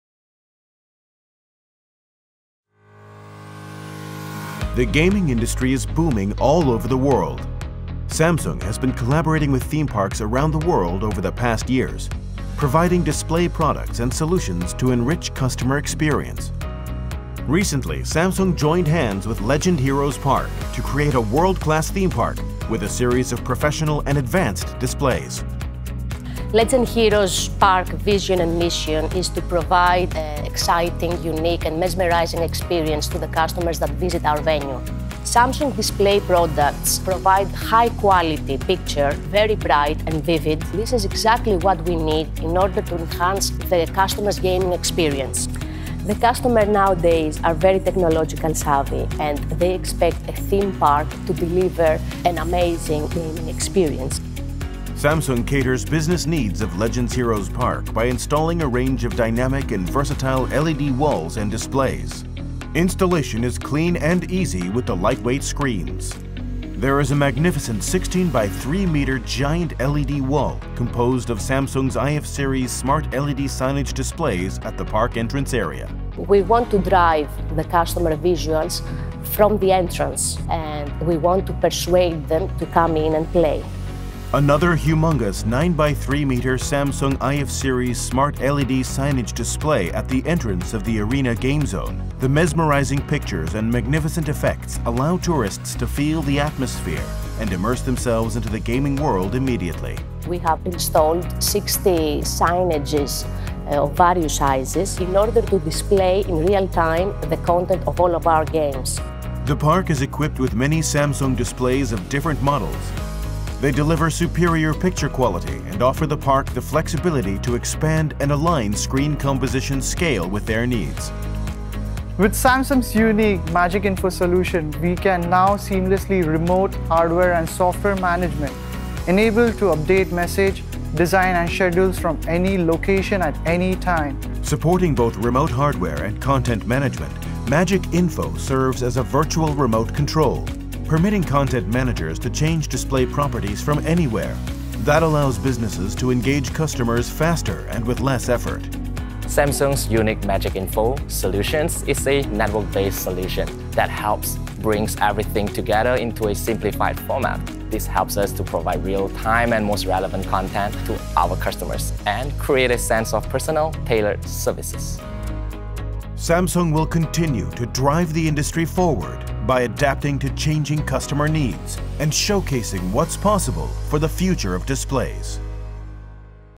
SMART Signage Solutions: Case Study - Macau Legend Heroes Park I Samsung
Legend Heroes Park is a technology-based entertainment park that showcases virtual and augmented reality, combining cutting-edge virtual technology with the physical world for an immersive experience.

To create an entertainment space that is as functional as it is technologically advanced, Legend Heroes Park chose an ideal partner in Samsung.